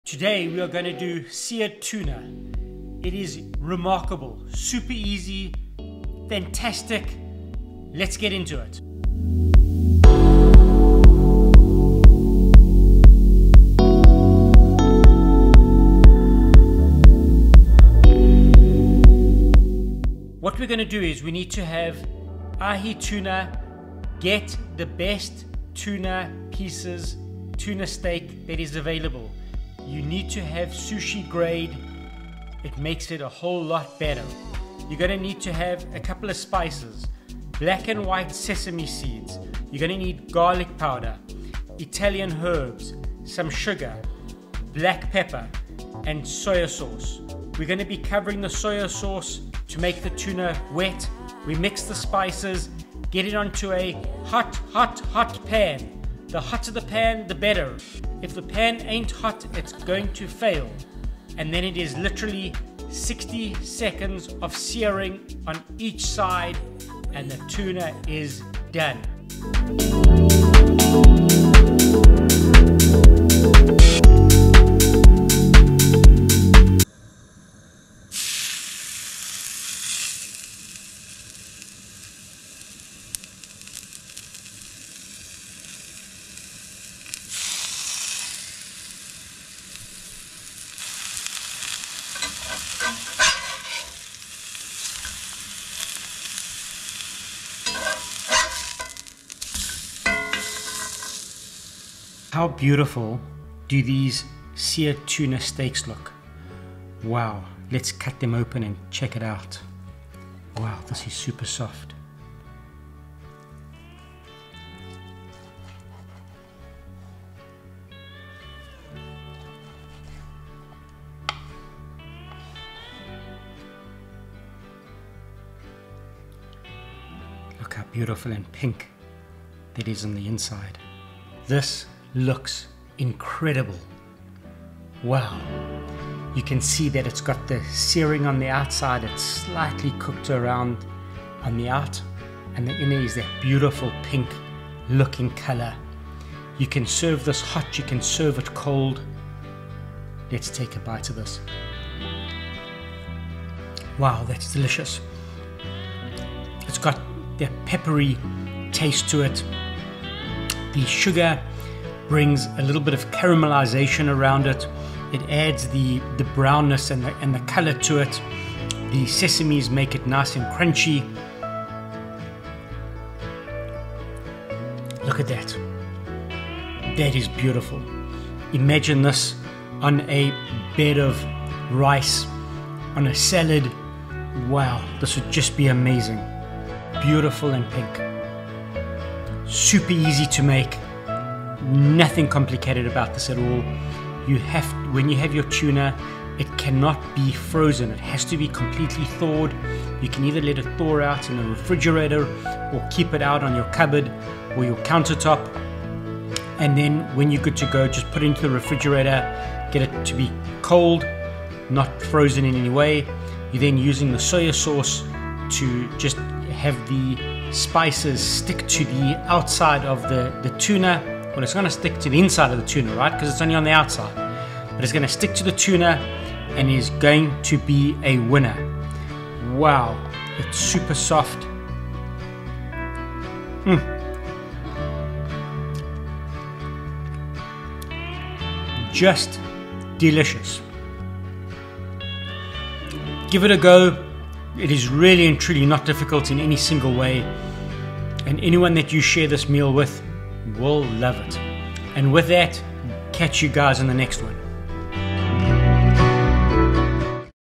Beginners guide to Seared Tuna | Hot or cold it's just awesome
Learn the secret to making the perfectly seared tuna with a sesame Seed crust in under 10 minutes. This tuna is a super healthy, delicious ahi tuna recipe packed with flavor and easy to make.  @MoaiFriends

Best tuna to use?
Whenever you serve rare Ahi tuna, use "Sushi Grade" tuna.

Process
Thaw the tuna (overnight in the refrigerator) or on the counter.
Make the spice crust.
3 Tablespoons Sesame Seeds (black and white)
1 Tablespoon Granulated Garlic
1 Tablespoon Crushed Black Pepper
1 Tablespoon Sugar
1 Tablespoon Italian Seasoning

2 Pieces of Tuna
1 Tablespoon of Soy Sauce
2 Tablespoons high heat oil

Coat the Ahi tuna in the sesame seed spice, pressing it into the flesh and coating all sides generously. The Soy Sauce helps it stick.
Heat the skillet, which needs to be very hot; add the oil. You can use any high-heat oil (I use 2 x Tablespoons Avocado oil).
Carefully place the Ahi tuna in the pan and turn it after 60 to 90 seconds; turn the Ahi tuna to the sides, and then remove from the pan.
The objective is to get a golden crust on all sides without cooking it all through.
Place the Ahi tuna on a cutting board and slice the tuna into thin pieces.
Serve either hot or cold.